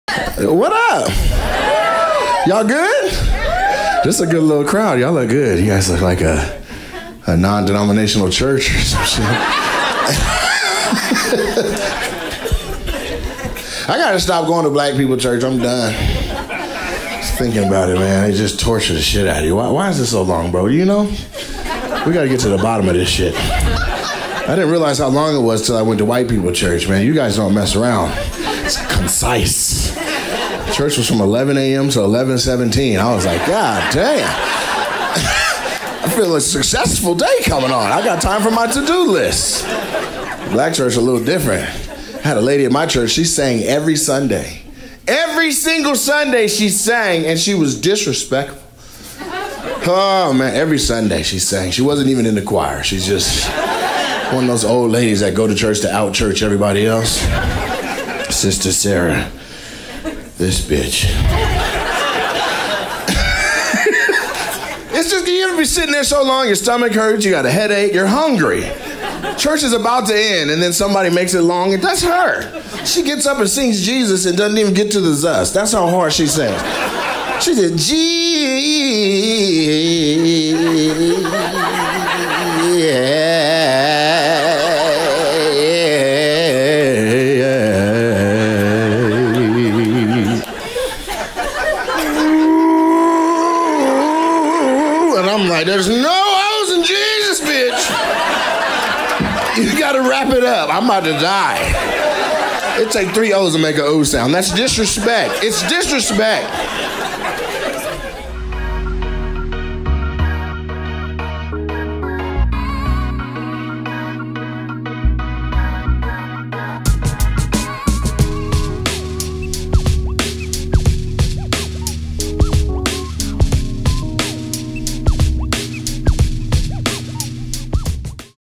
Nate Jackson | Church Lady | Laugh Factory Las Vegas Stand Up Comedy
Nate (@mrnatejackson) thinks some church ladies and are just in it for the vocal exercises. See a LIVE SHOW at the Hollywood Laugh Factory!